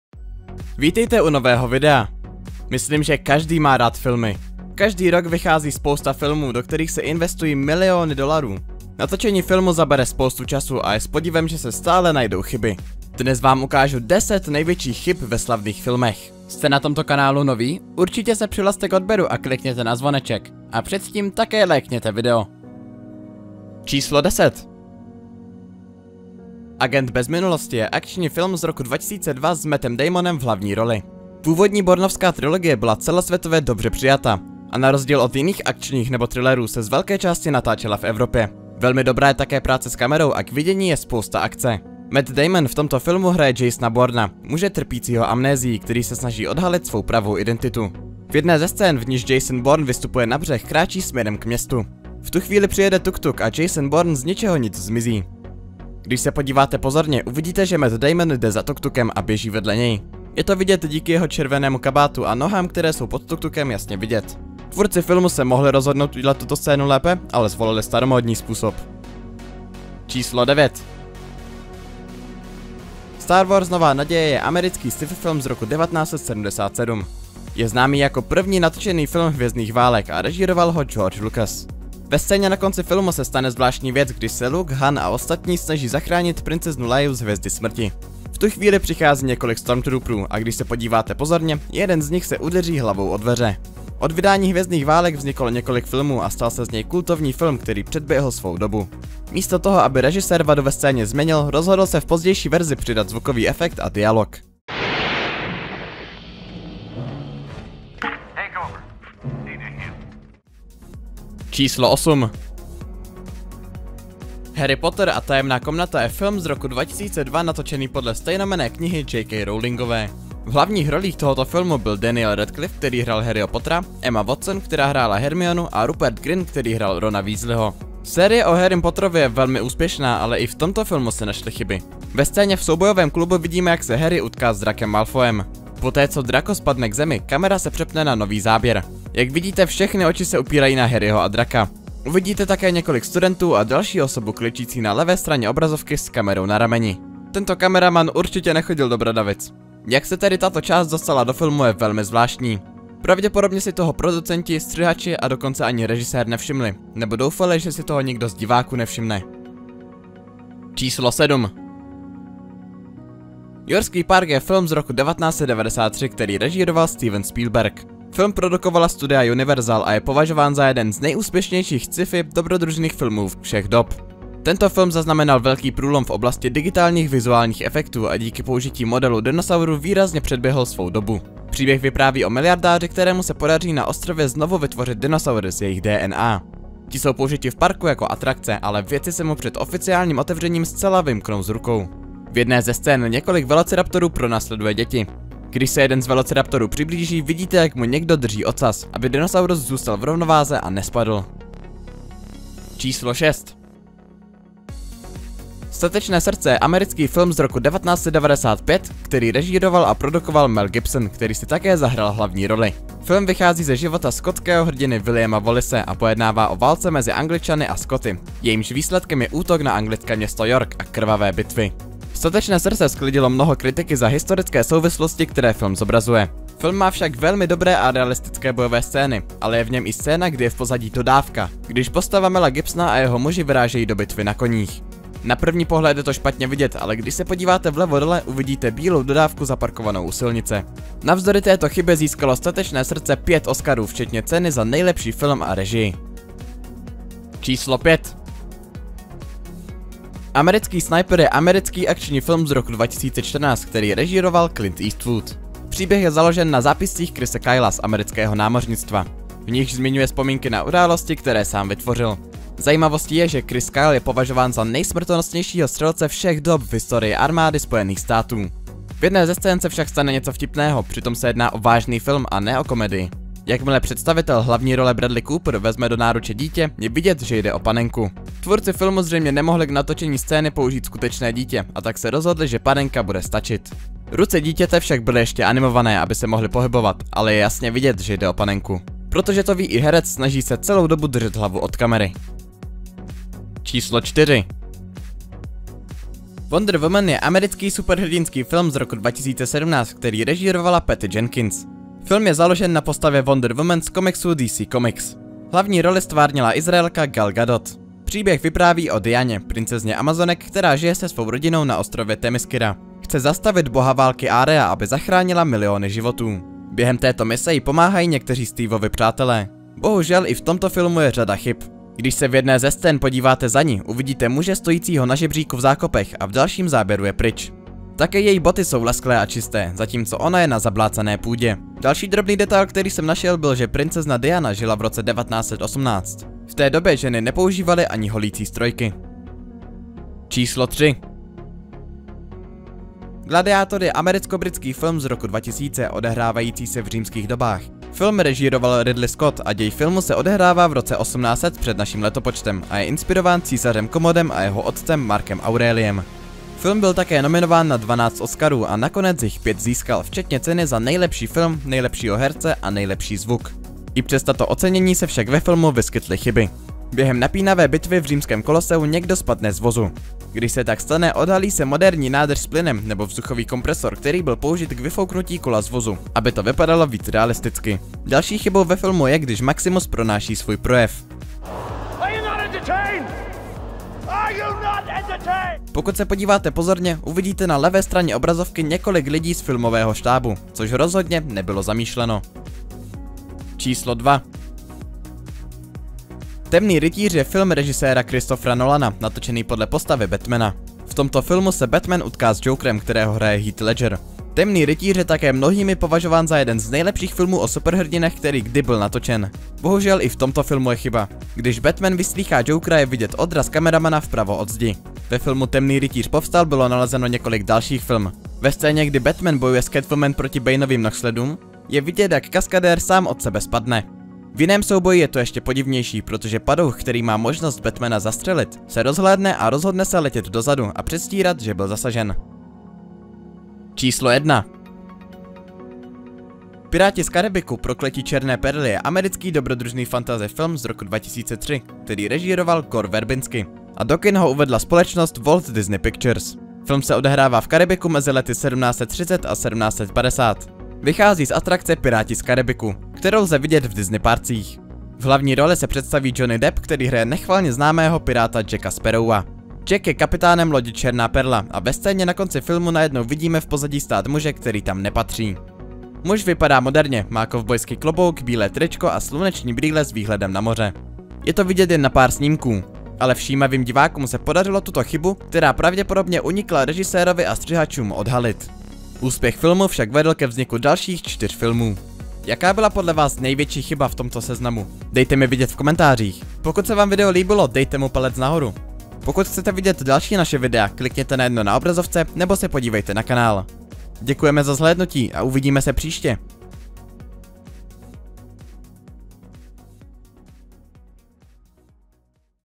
10 Největších Chyb ve Slavných Filmech
Každý se rád dívá na filmy a každý rok se objeví spousta filmů, které stojí miliony dolarů. Natočení filmu zabere spoustu času a je překvapivé, že se stále najdou chyby. Dnes vám ukážeme 10 slavných filmů, ve kterých se objevily velké chyby, kterých si producent nevšiml.